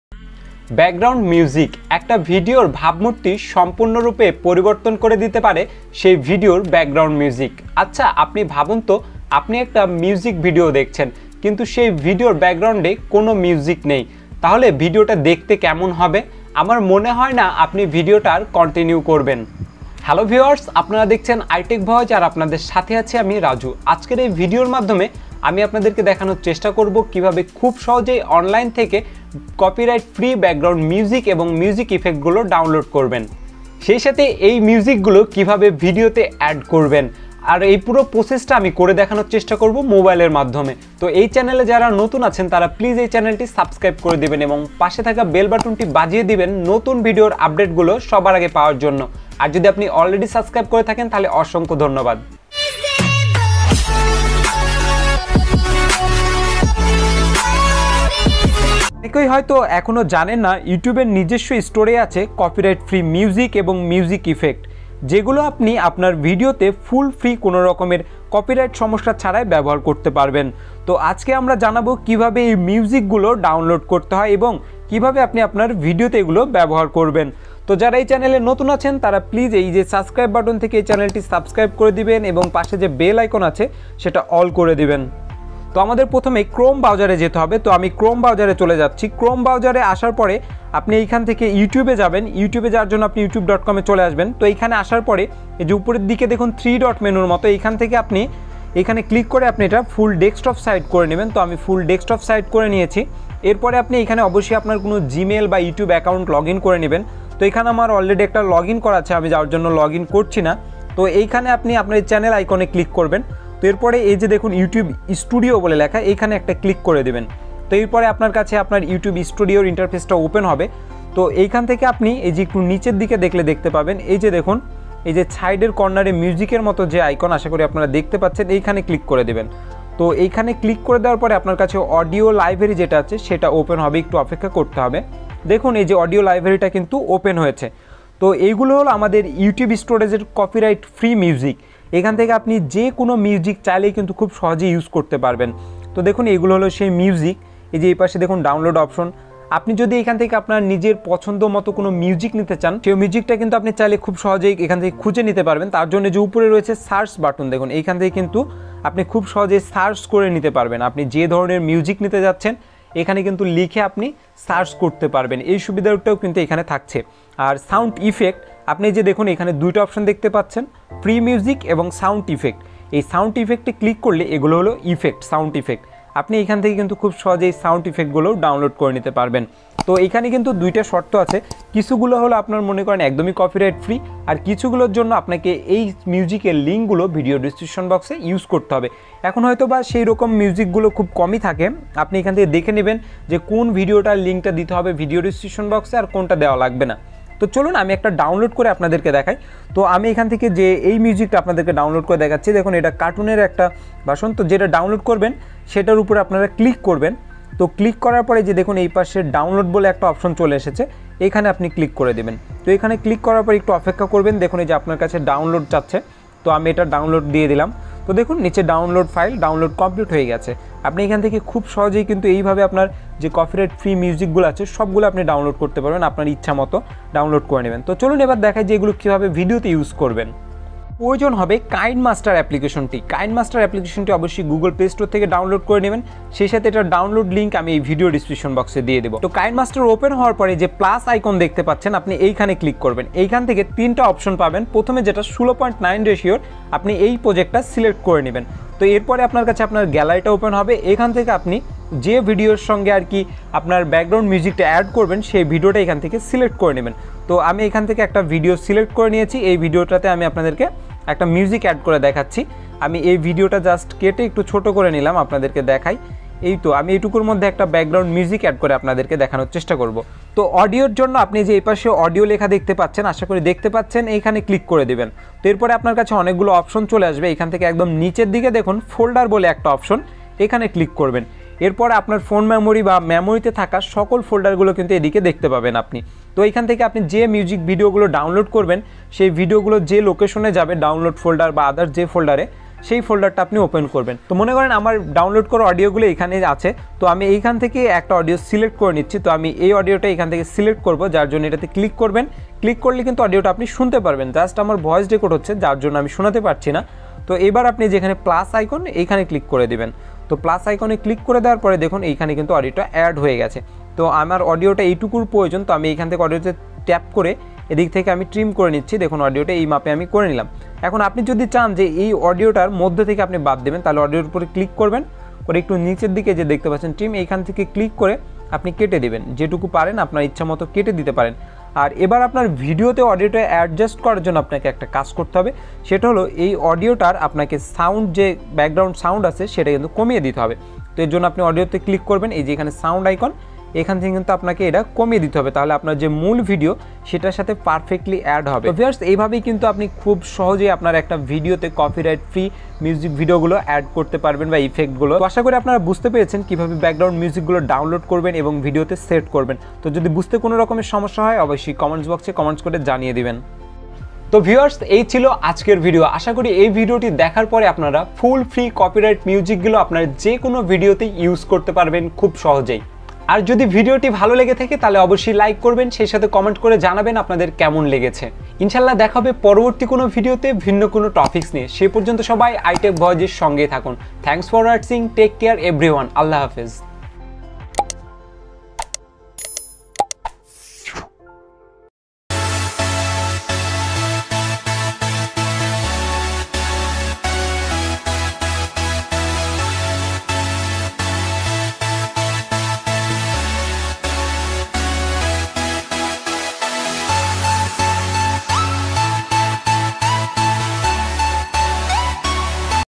ফ্রী ব্যাকগ্রাউন্ড সাউন্ড | Copyright Free Background Music & Sound | iTECH VOICE
This video I will show how to download copyright free background music and sound effects, and I also show how to add background sound and music effect in a video on android mobile bangla. YouTube have own sound storage. In this storage you can find many copyright free music and sound effect. For add background music I used Kinemaster app. You find this app for Android or Apple. I tried to easily explain everything to you.

এই ভিডিওতে আমি আপনাদেরকে দেখিয়েছি কিভাবে কপিরাইট ফ্রি ব্যাকগ্রাউন্ড মিউজিক এবং সাউন্ড ইফেক্ট ডাউনলোড করবেন এবং কিভাবে এগুলো আপনার ভিডিওতে সেট করতে পারবেন মোবাইল এর মাধ্যমে। আশা করি ভিডিওটি দেখে আপনাদের ভালো লাগবে।

➡️➡️Download Link